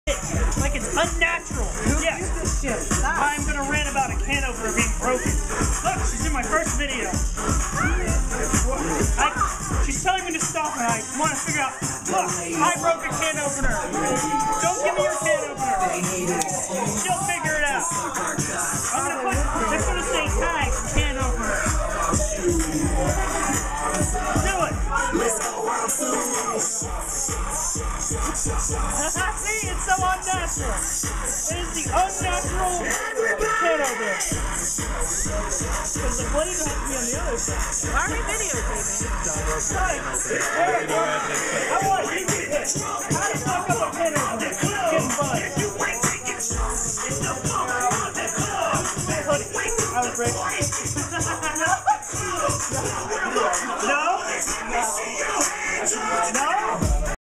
Friends + Me trying to put a can opener back together
Friends + Me trying to put a can opener back together. My new keyboard is green like HULK